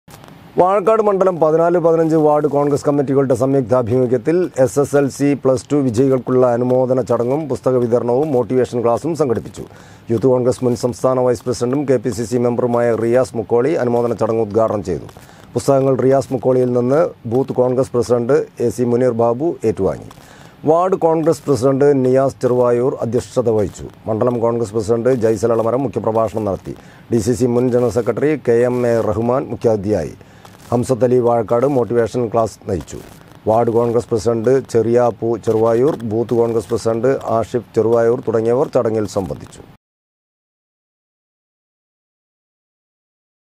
വാഴക്കാട് മണ്ഡലം 14 ,15 വാർഡ് കോൺഗ്രസ് കമ്മിറ്റികളുടെ സംയുക്താഭിമുഖ്യത്തിൽ എസ്.എസ്.എൽസി, പ്ലസ് ടു
വാഴക്കാട് മണ്ഡലം 14 ,15 വാർഡ് കോൺഗ്രസ് കമ്മിറ്റികളുടെ സംയുക്താഭിമുഖ്യത്തിൽ എസ്.എസ്.എൽസി, പ്ലസ് ടു വിജയികൾക്കുള്ള അനുമോദന ചടങ്ങും, പുസ്തകവിതരണവും, മോട്ടിവേഷൻ ക്ലാസും സംഘടിപ്പിച്ചു.